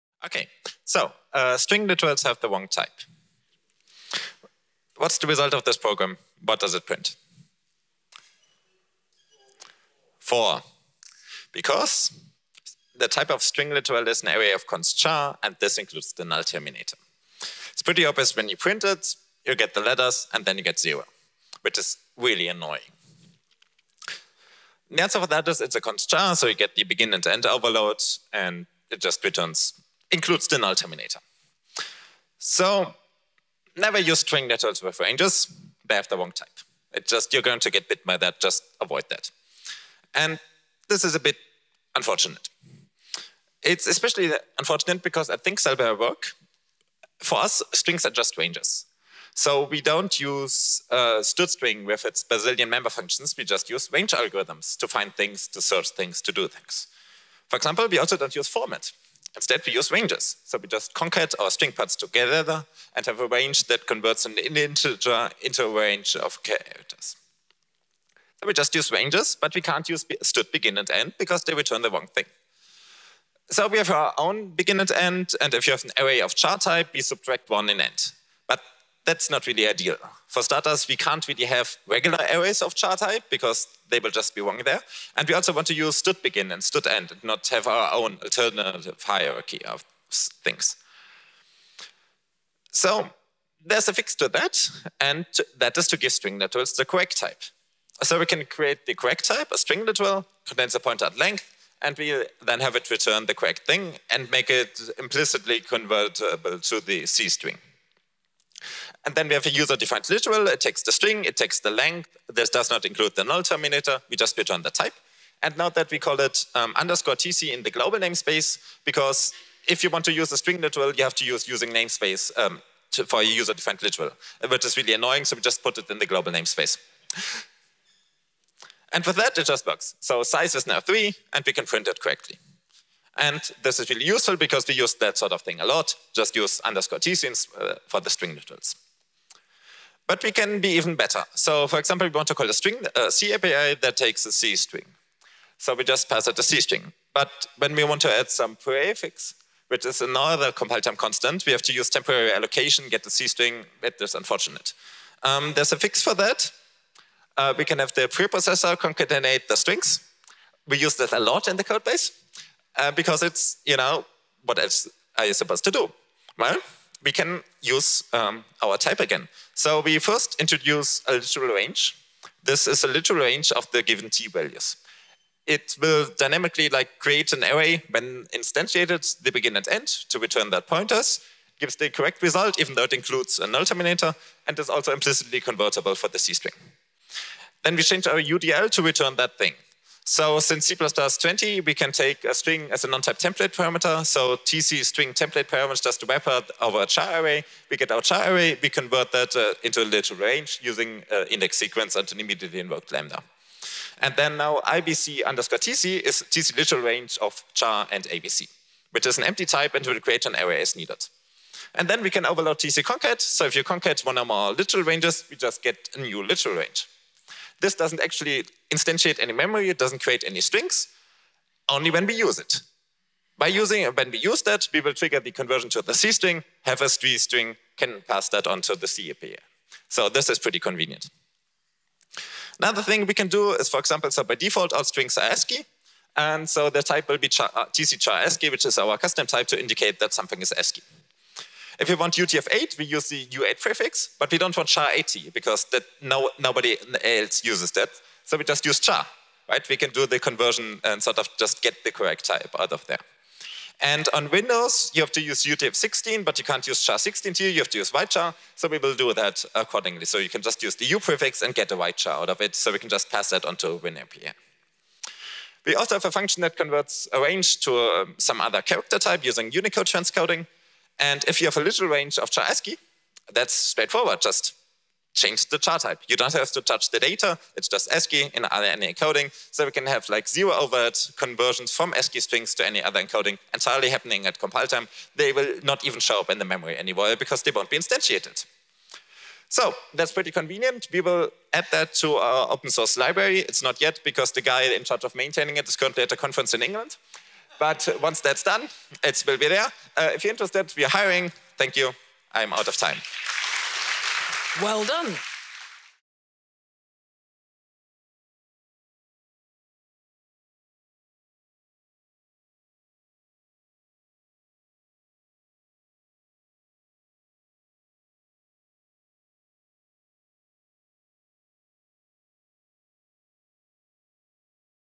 Lightning Talk: C++ String Literals Have the Wrong Type - Jonathan Müller - C++ on Sea 2023
Lightning Talk: String Literals Have the Wrong Type - Jonathan Müller - C++ on Sea 2023

`std::ranges::size("abc")` is `4`, not `3` - it includes the trailing null-terminator. We can fix that with UDLs, NTTPs and TMP.

Jonathan Müller

Jonathan is a library developer at think-cell. In his spare time, he works on various C++ open source libraries for memory allocation, cache-friendly containers, or parsing. He also blogs at foonathan.net and is a member of the C++ standardization committee.

C++ on Sea is an annual C++ and coding conference, in Folkestone, in the UK.